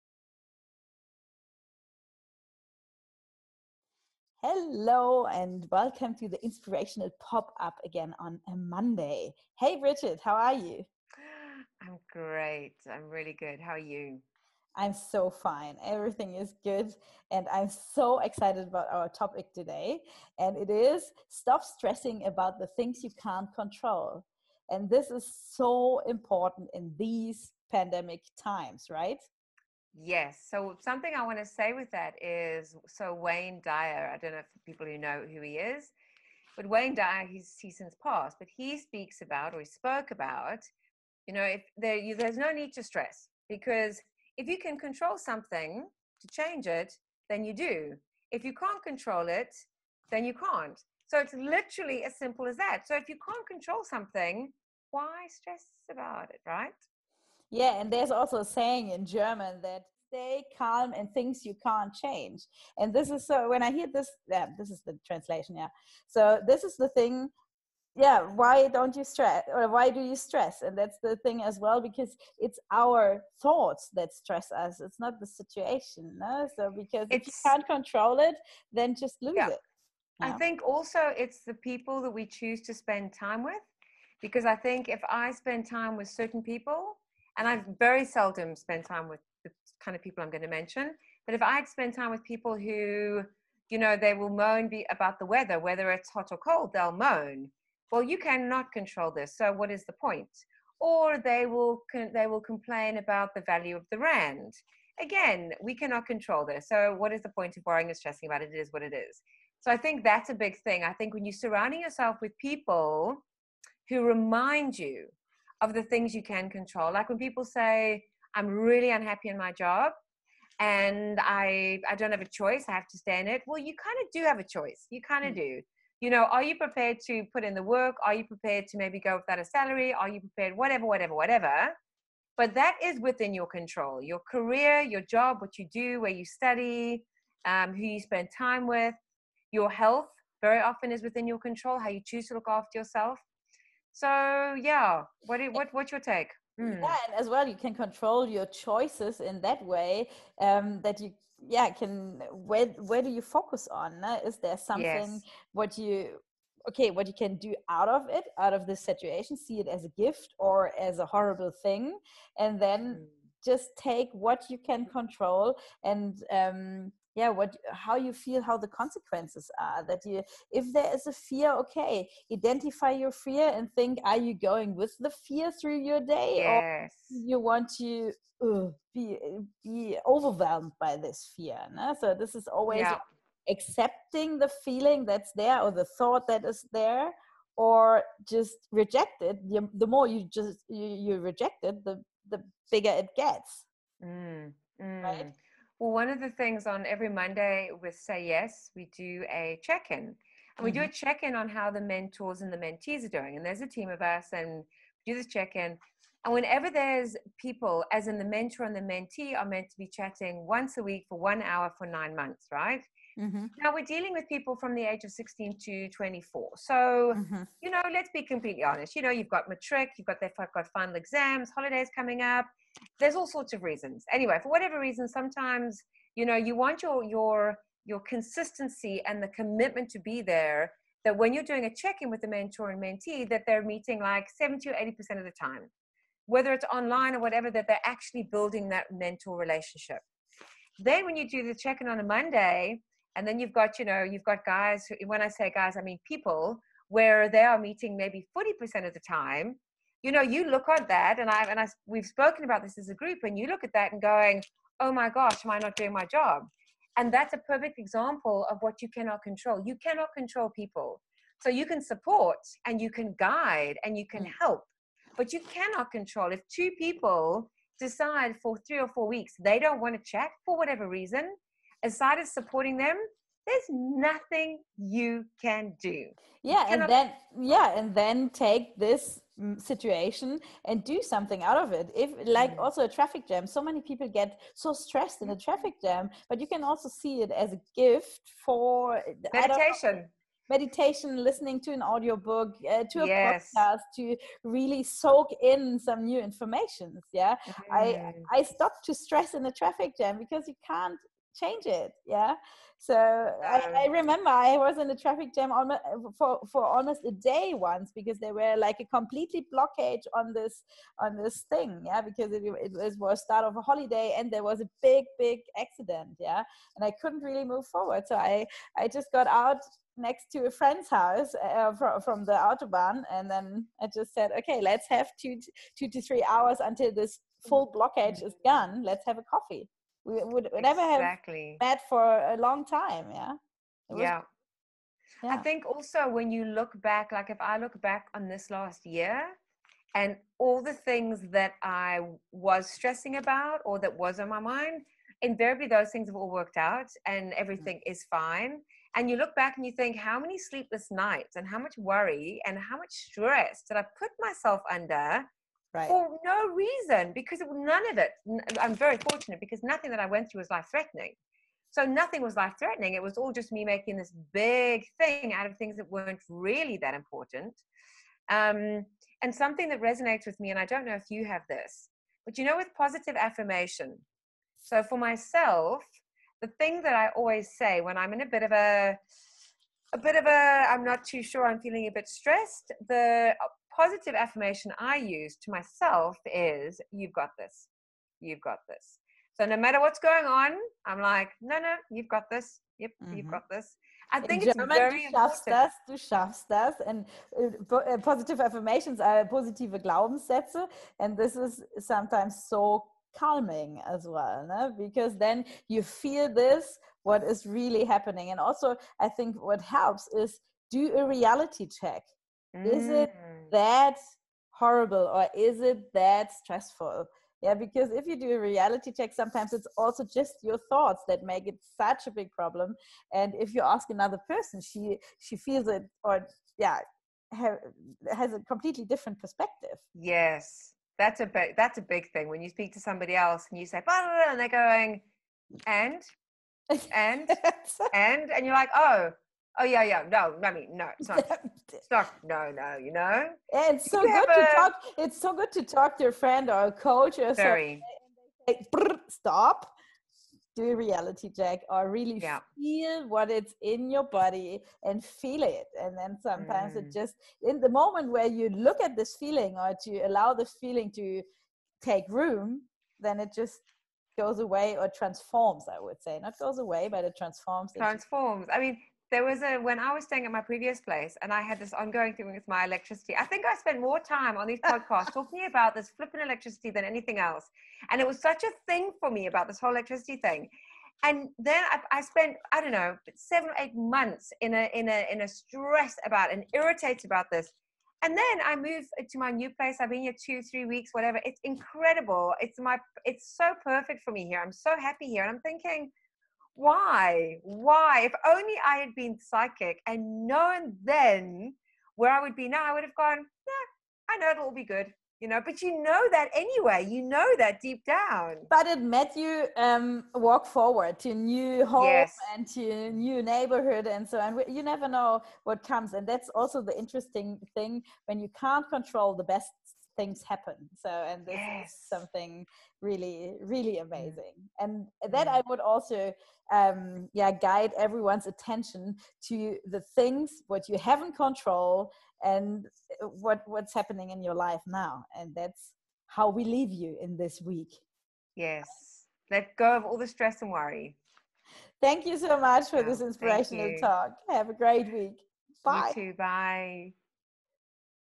Stop Stressing @Shift1degree @TravelinsideINSPIRATIONSCOACH · Personal Coach
Acknowledge that your thoughts aren't helpful, and get up and go do something else for a few minutes to get your brain focused on something more productive.